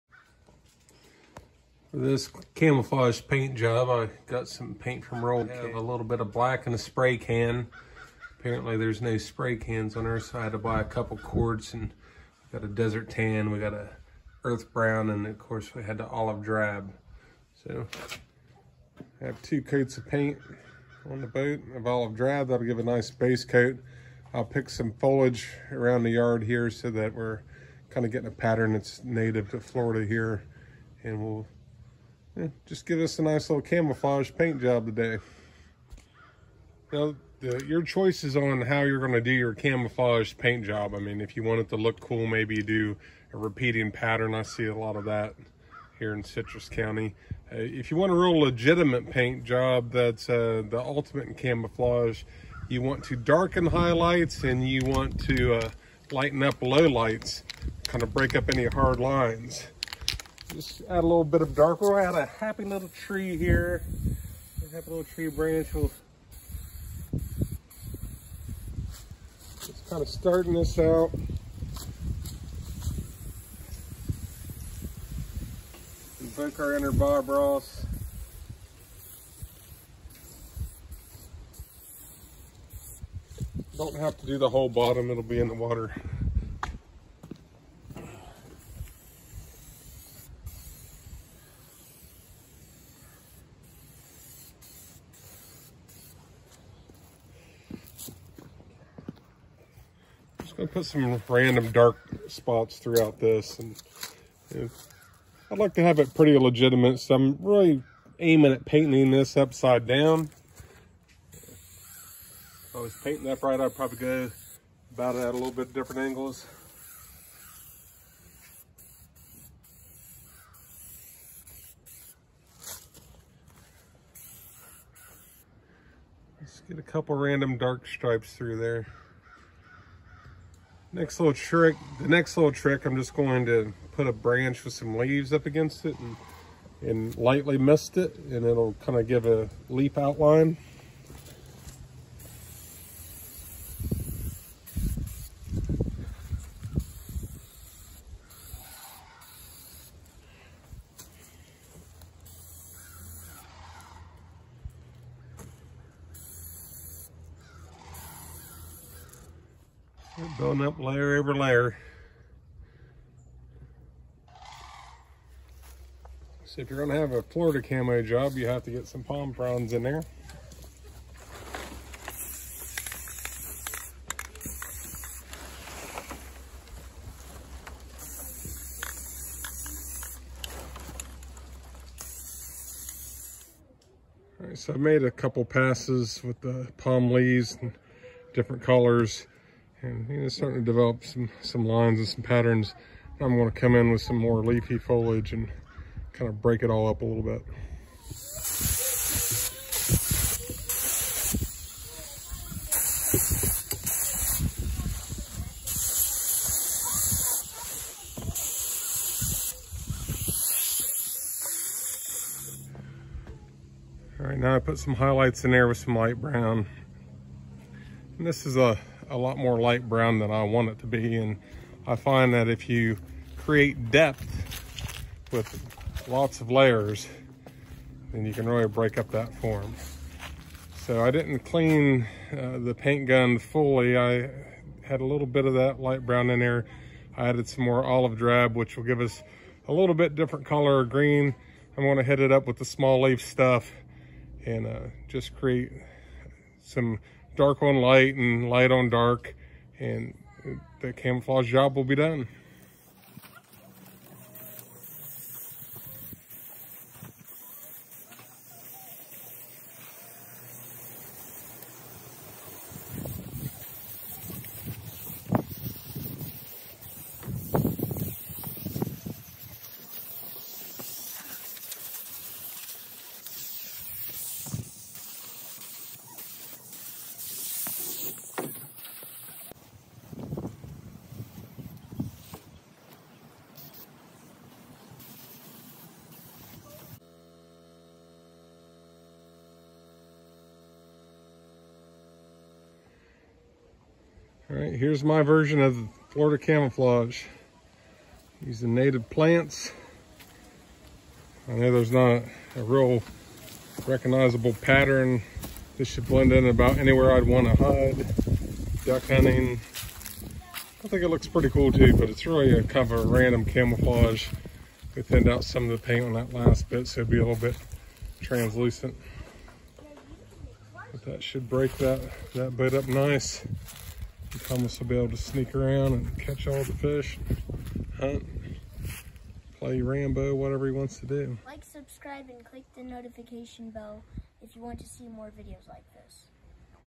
Budget Boat Build PT 4 Camouflage Paint Job
Beau finishes the hull paint in camouflage using foliage from the yard. The base layer was rolled on. It is easy do create a nice camo job with spray paint. With supply shortages Beau bought pints of paint and used a cheap Harbor Freight gun to lay down the paint. Beau demonstrates how to layer palm fronds branches, and vines to breakup the hard lines on the hull.